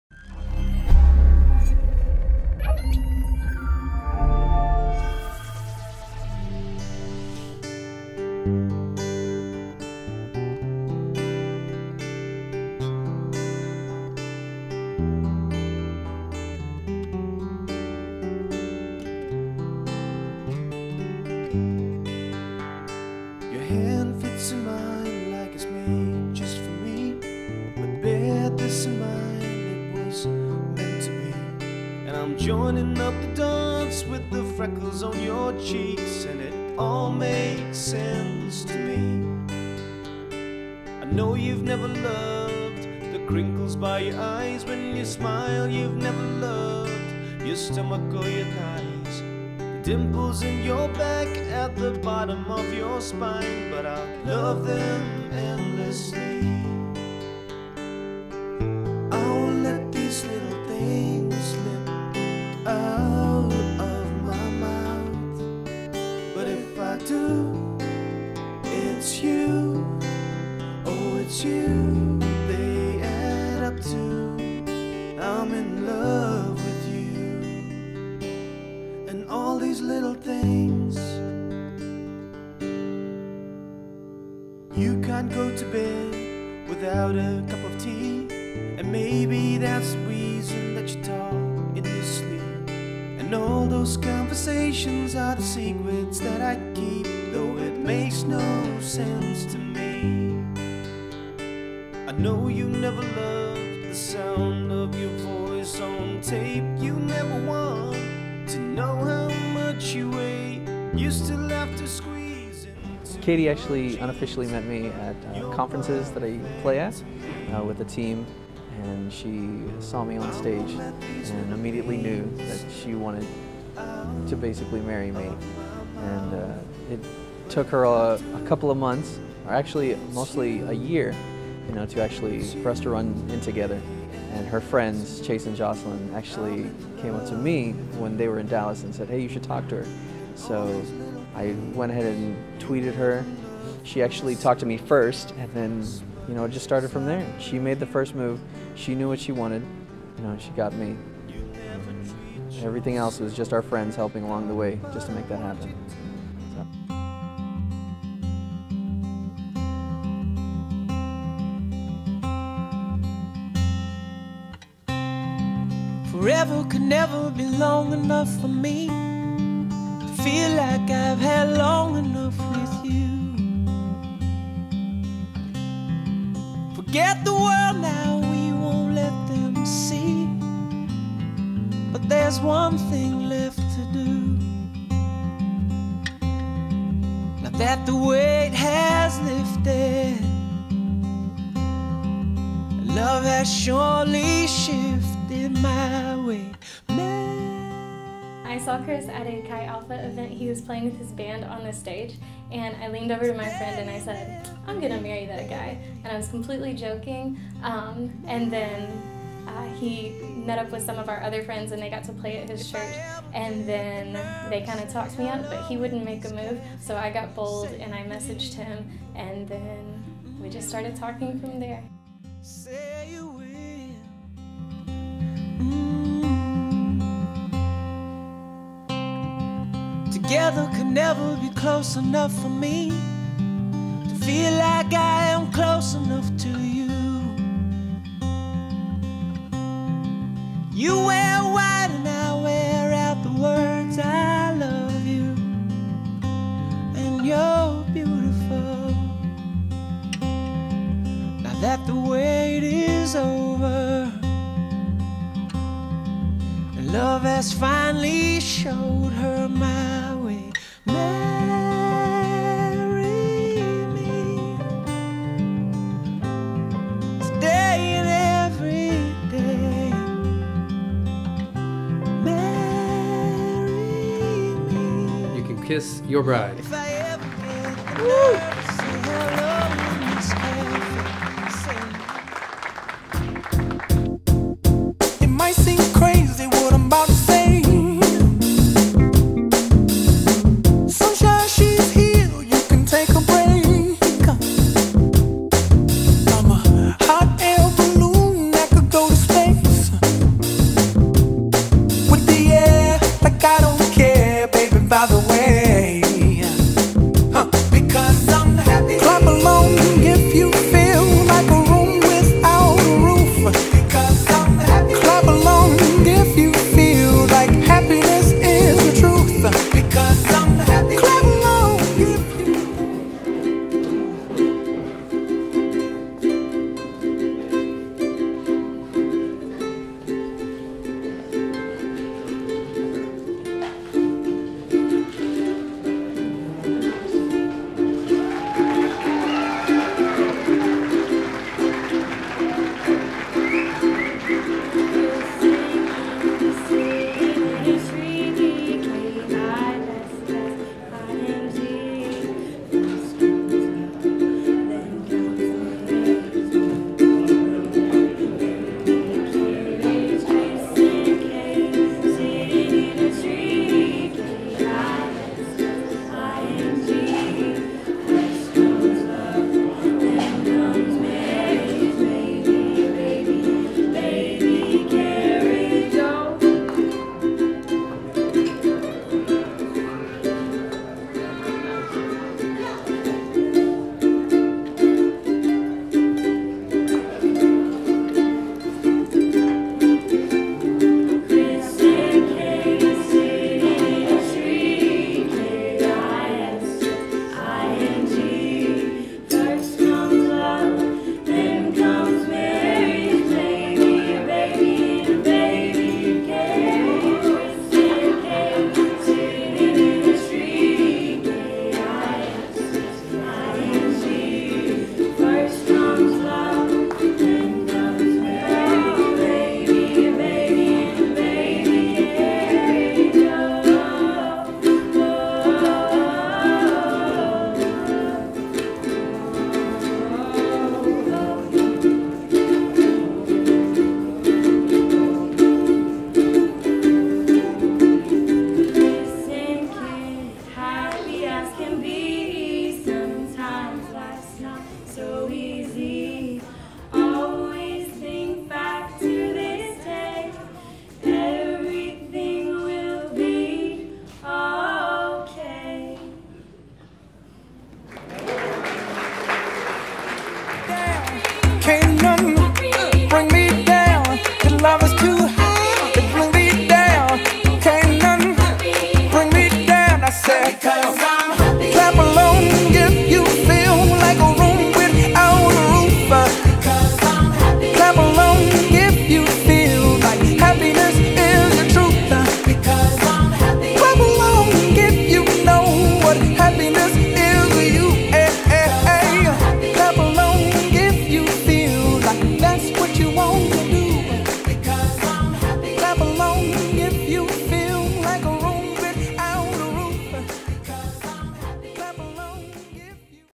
The Rixon's - Wedding film 2014
We love this awesome God fearing couple. They are perfect for each other! This is their wedding. Blessed to be able to capture this very memorable moment!